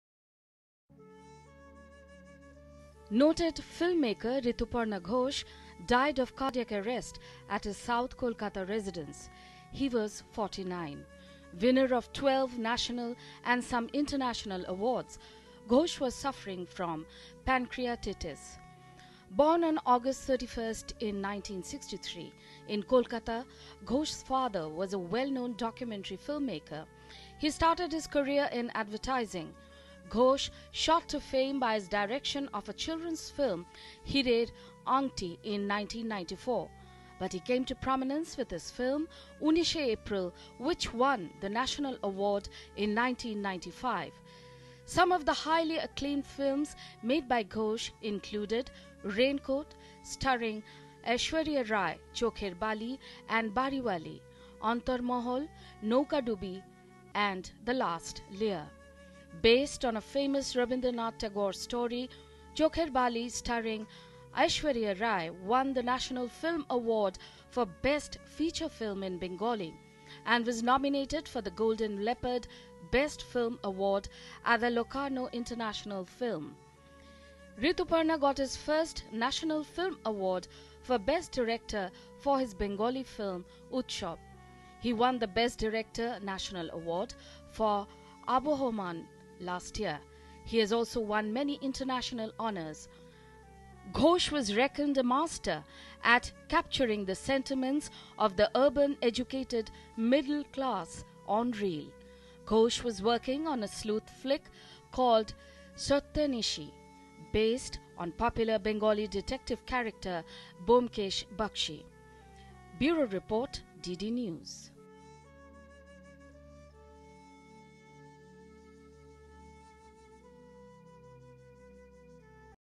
Filmmaker Rituparno Ghosh passes away in Kolkata of cardiac arrest
Award winning Bengali filmmaker Rituparno Ghosh passed away in Kolkata of cardiac arrest. He was 49 years old. He had won several national awards and several international awards.Last year, he got the Best Director National Award for his Bengali film 'Abohoman'. Rituparno Ghosh is credited for helming many other critically acclaimed and National award-winning films like Dahan, Utsab, Chokher Bali, Raincoat, Dosor, The Last Lear, Shob Charitro Kalponik.Ghosh was born on August 31, 1963 in Kolkata. His father was a well known documentary filmmaker.Ghosh came to prominence with his 1994 film 'Unishe April'.